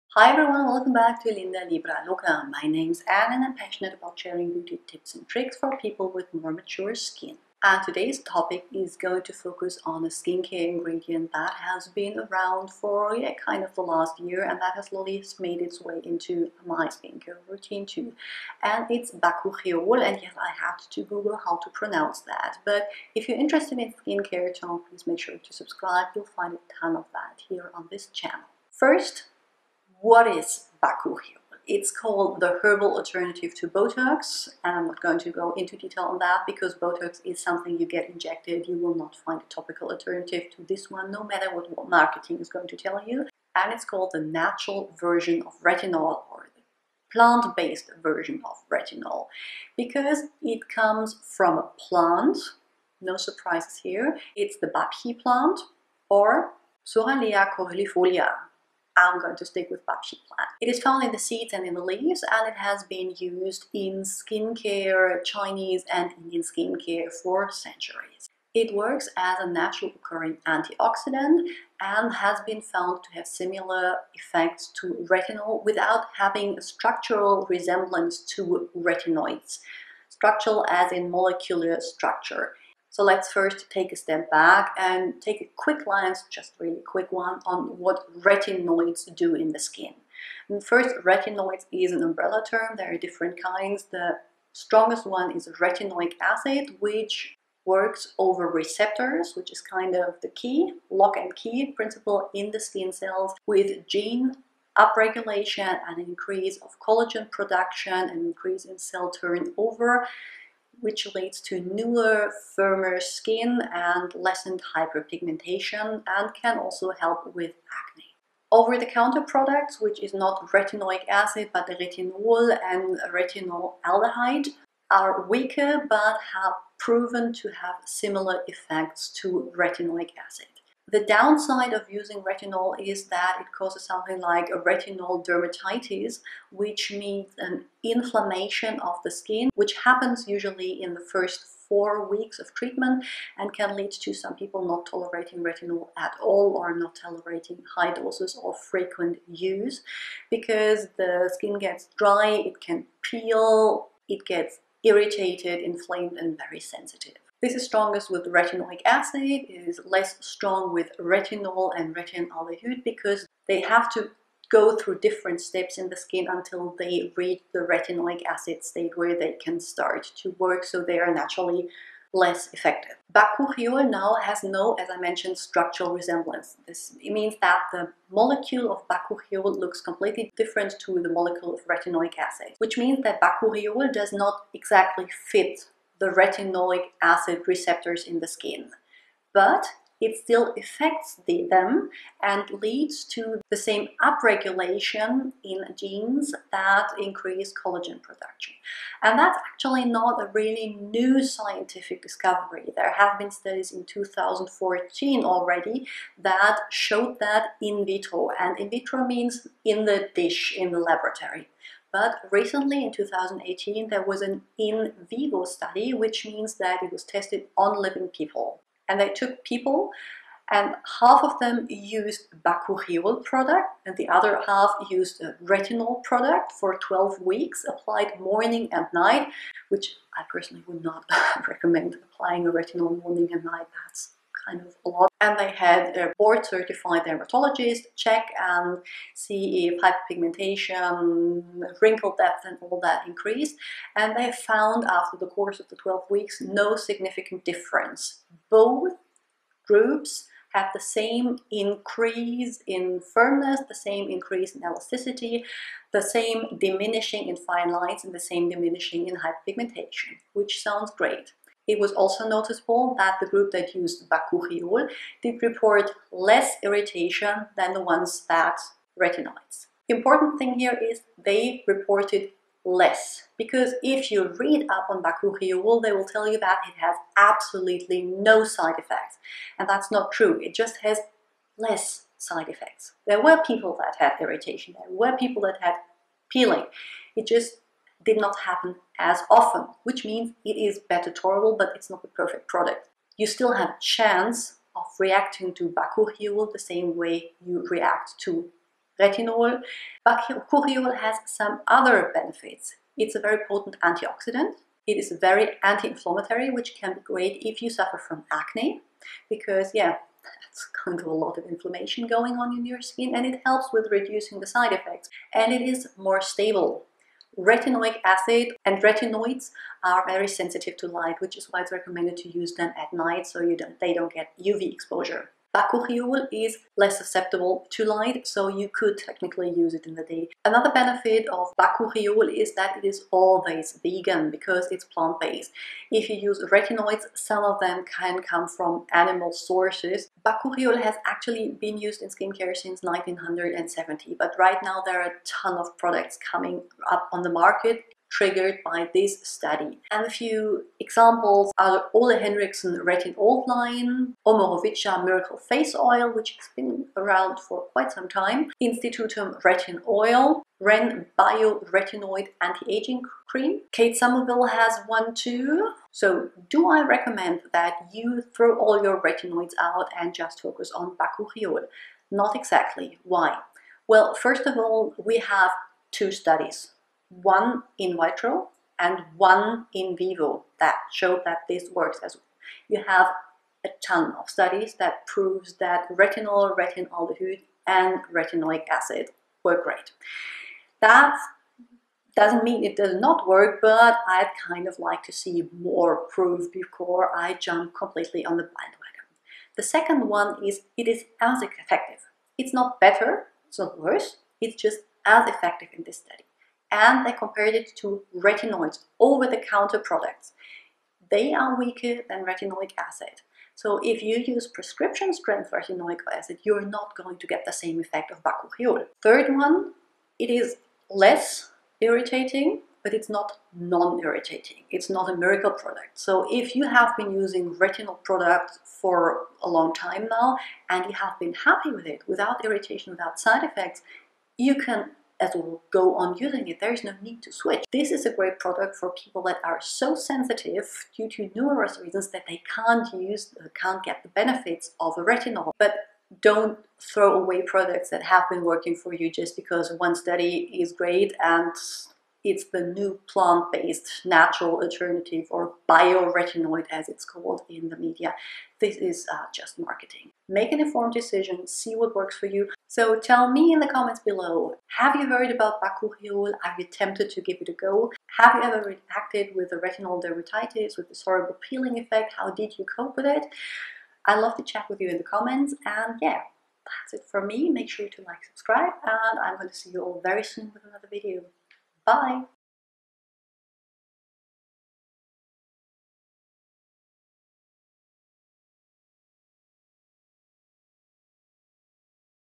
Bakuchiol vs Retinol - Which one is best for you? | Doctor Anne
You might have heard of the new super ingredient Bakuchiol. Or by its many other names like Vegan Retinol, Bio Retinol, natural alternative to Retinol, Retinol without irritation. So let´s look at Bakuchiol vs Retinol and find out which one is best for you.

Ole Henriksen Retin-Alt
Omorovicza Miracle Face Oil
Instytutum RetinOil*
Oskia Super 16 Serum
REN Bio Retinoid Anti-Aging Cream
Kate Somerville DermalQuench Liquid Lift

My equipment:
Camera: Nikon DSLR D7200

All products with the exception of those marked with an (*) were purchased with my own money and all opinions are honest and my own.